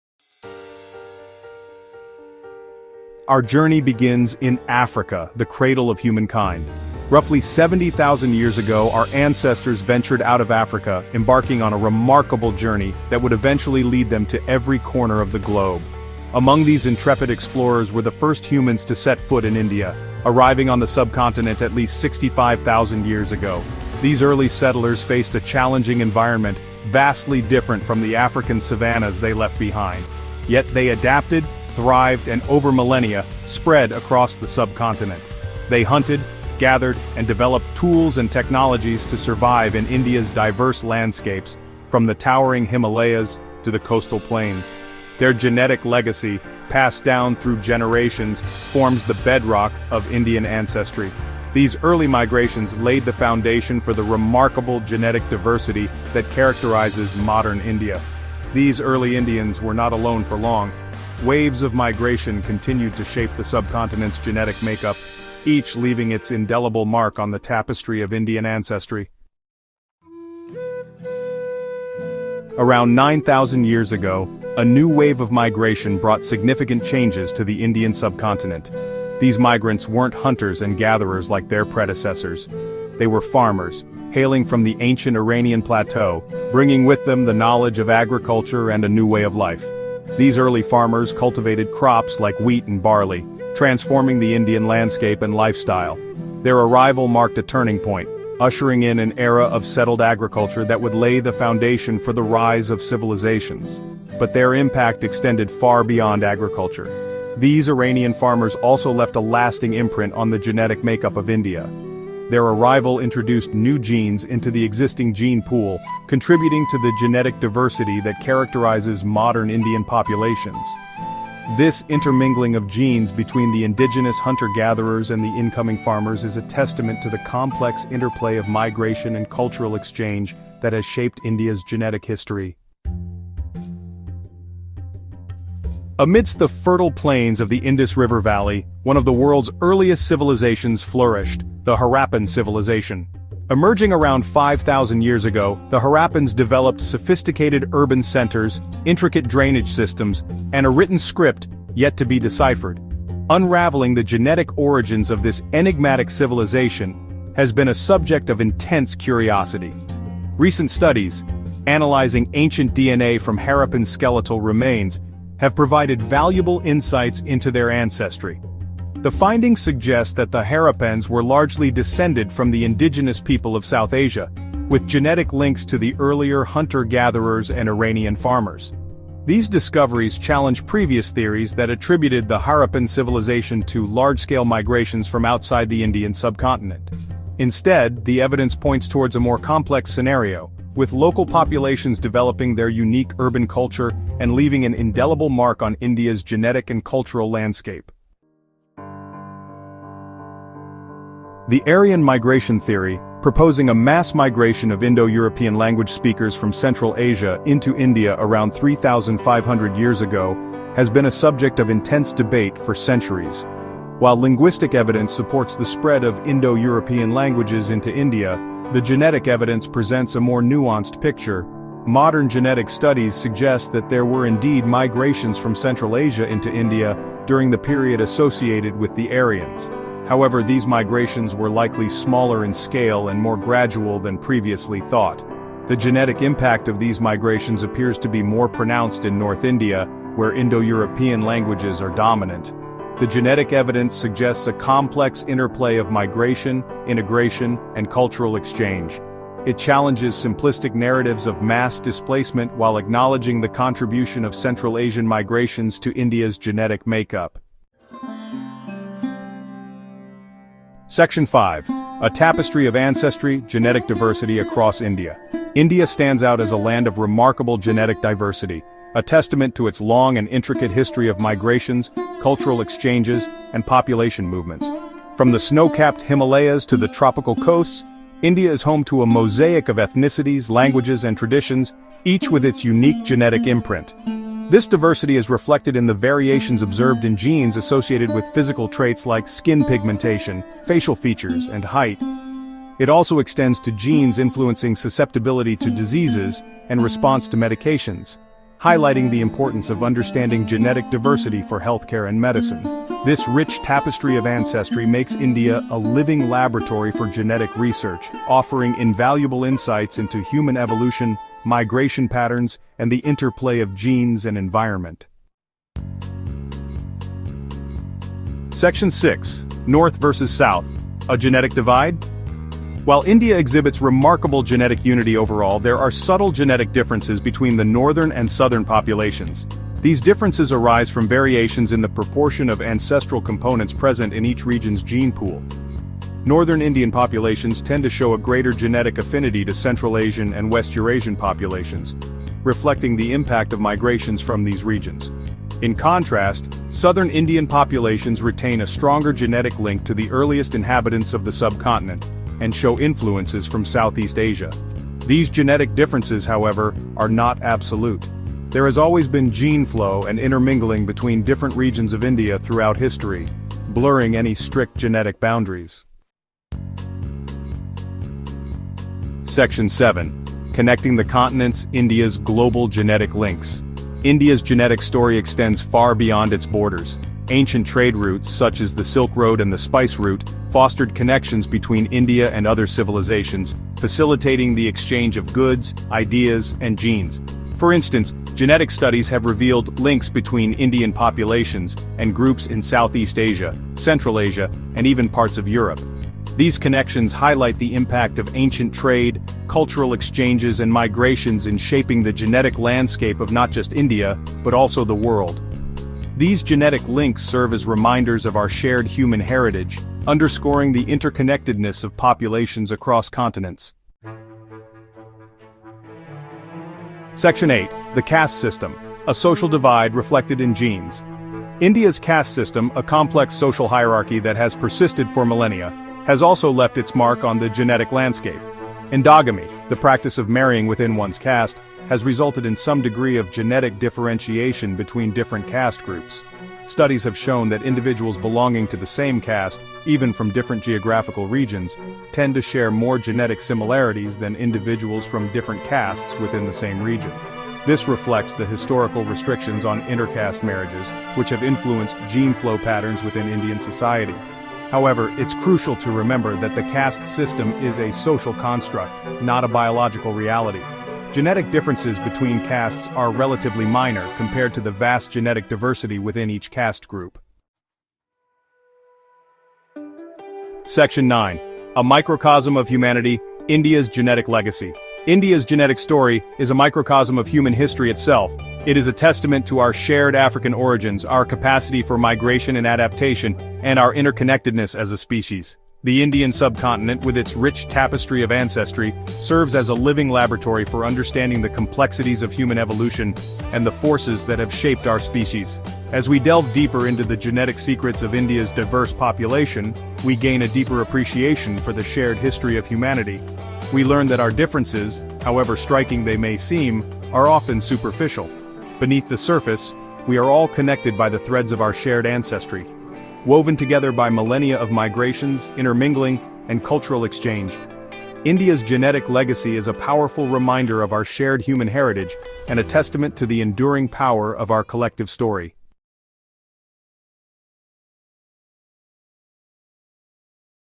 Scientists Reveal Surprising Genetic Origins of INDIA
Dive into the astonishing genetic tapestry of India in our latest video, "Unveiling India's Genetic Secrets: A Journey Through Time." Explore how groundbreaking DNA research reveals a complex history of ancient migrations and lost civilizations, reshaping our understanding of one of the world’s oldest cultures. From the first humans leaving Africa to the blending of indigenous tribes with ancient Iranian farmers and steppe pastoralists, discover how these interactions have shaped India's diverse populations today.

Join us on this captivating journey through time, and witness how genetics tells the story of humanity's shared legacy.

OUTLINE:

00:00:00 The First Indians
00:01:09 A Genetic Revolution
00:02:12 Unlocking Harappan Genes
00:03:18 Separating Myth from Reality
00:04:17 Genetic Diversity Across India
00:05:15 A Genetic Divide?
00:06:06 India's Global Genetic Links
00:06:53 A Social Divide Reflected in Genes?
00:07:47 India's Genetic Legacy